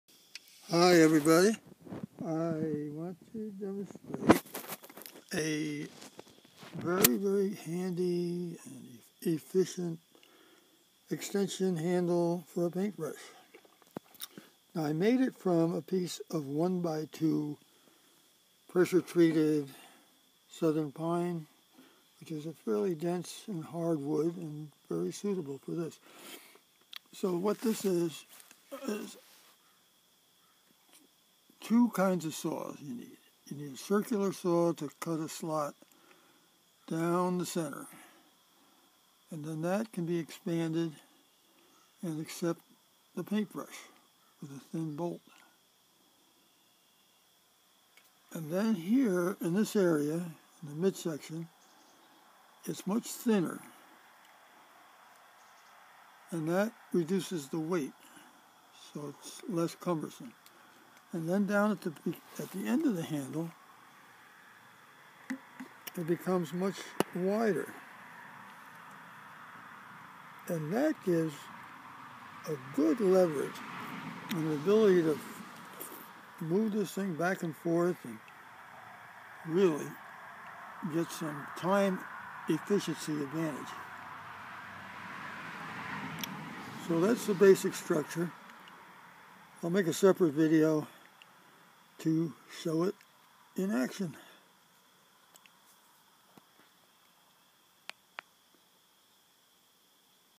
How to make a paint brush extension handle to greatly simplify painting.
The added length means fewer ladder moves as well as no bending to paint floors and only a one foot step-up to do ceilings.